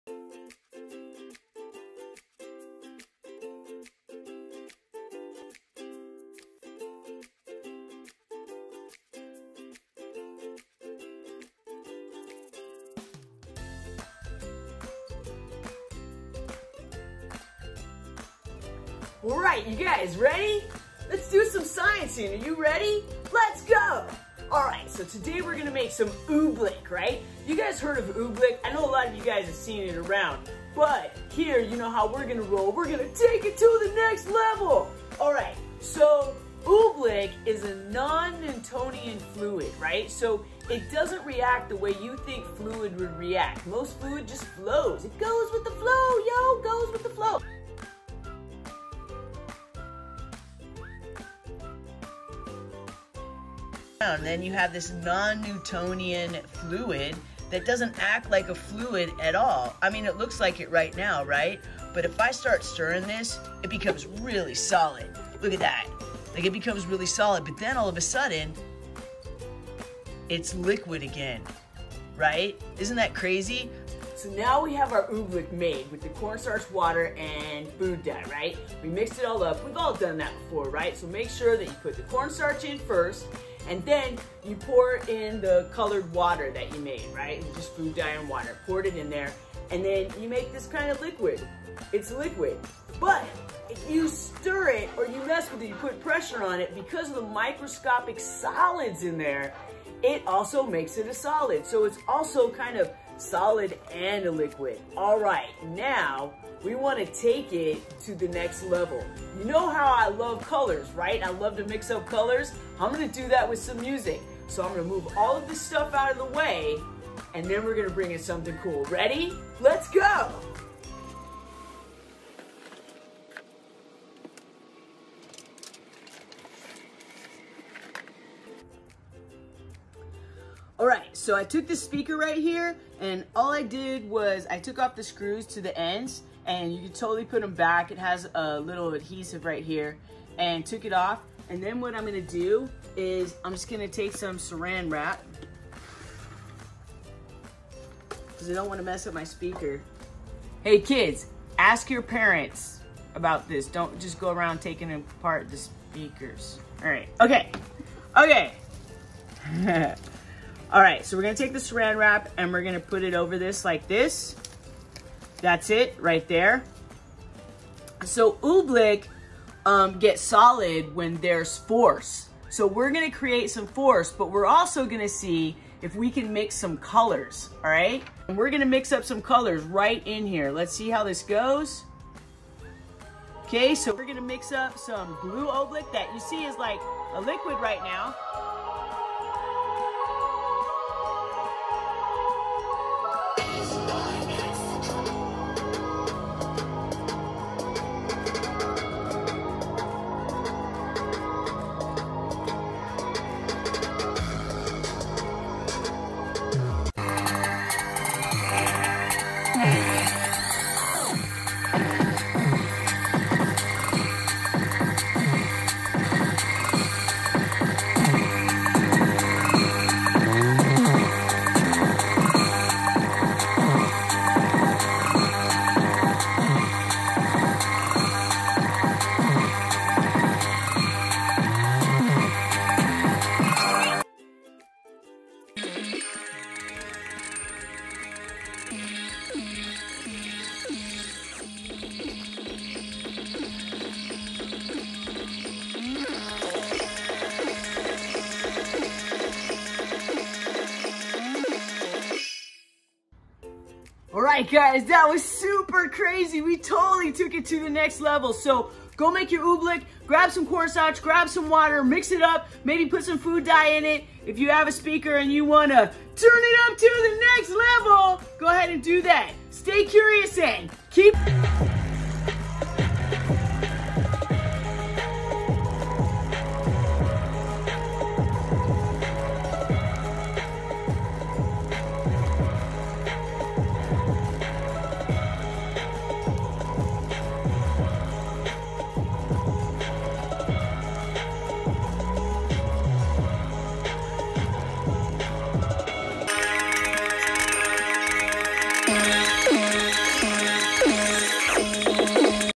Oobleck Easy Kid Science hertz speaker
Easy science experiment for kids. Oobleck a Non Newtonian Fluid ( not following Newton’s law of viscosity ) - Corn Starch, Water, food dye - on a speaker with 80-100 Hertz